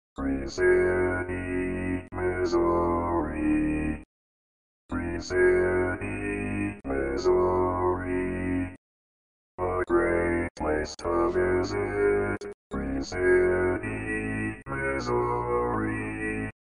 How to Say or Pronounce USA Cities — Green City, Missouri
This video shows you how to say or pronounce Green City, Missouri.

A computer said Green City, Missouri. How would you say Green City, Missouri?